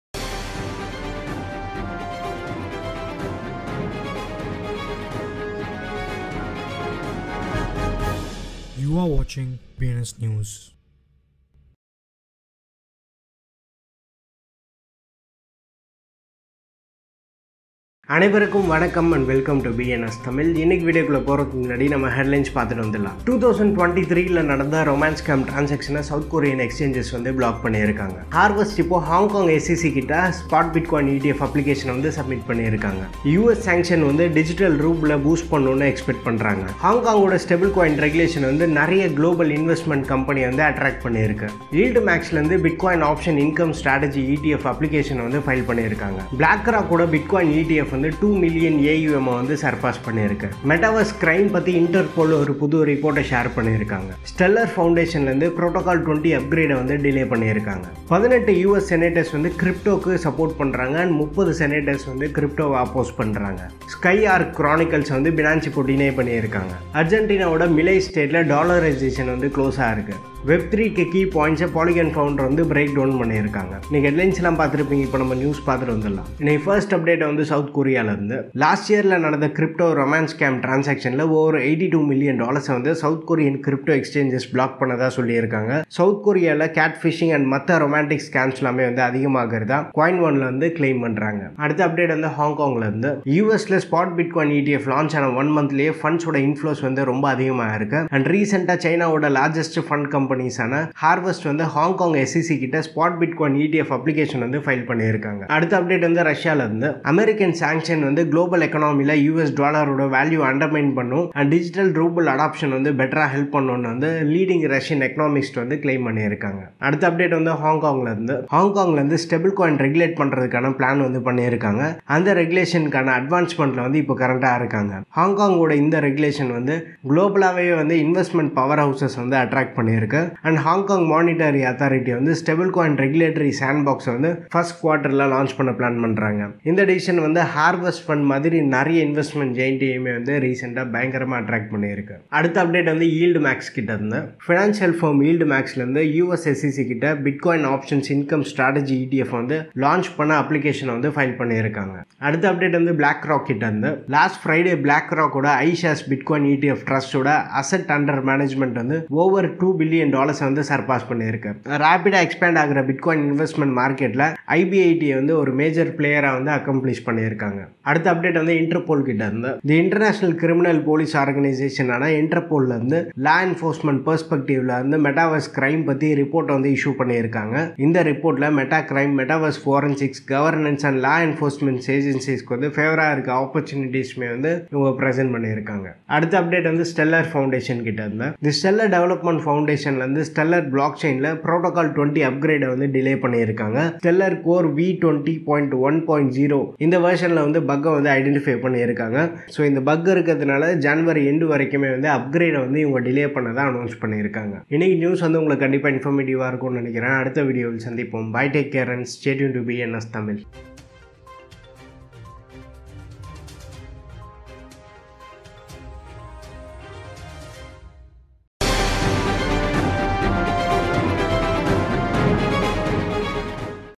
30/01/2024 Cryptocurrency Tamil news today | Shiba inu coin news | luna crypto news | Bitcoin Tamil
30/01/2024 Cryptocurrency news today Tamil | இன்றைய க்ரிப்டோ செய்திகள் | luna crypto news Tamil

- South Korean Crypto Exchanges Blocked $82m Worth of Romance Scam Transactions in 2023
- Chinese Fund Giant Harvest Submits Spot Bitcoin ETF Application to Hong Kong SEC
- US Sanctions Will Undermine Dollar and Boost Digital Ruble
- Hong Kong’s Race to Regulate Stablecoins Attracts Global Investment Heavyweights
- YieldMax Files Application For Bitcoin Option Income Strategy ETF
- BlackRock’s Bitcoin ETF Surpasses $2 Billion AUM, Market Remains Uncertain
- Interpol Publishes Report on Crime in the Metaverse
- Stellar foundation urges delay of Protocol 20 upgrade after bug discovery
- "Stand With Crypto” Lobby Group Says 18 US Senators are Pro-Crypto
- Binance Labs Denies SkyArk Chronicles’ Funding Claims
- Argentina’s Milei States Dollarization Is Close, Bitfarms Expands In Paraguay
- Polygon Founder Breaks Down Key Points for Web3 to Attain ‘Internet-Level’ Success